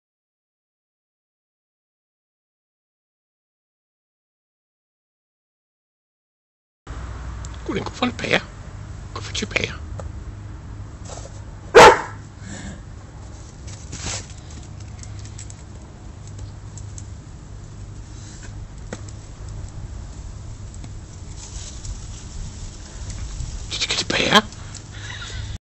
Boris the Doberman takes a pear
Boris my Doberman takes a pear from a tree Part Two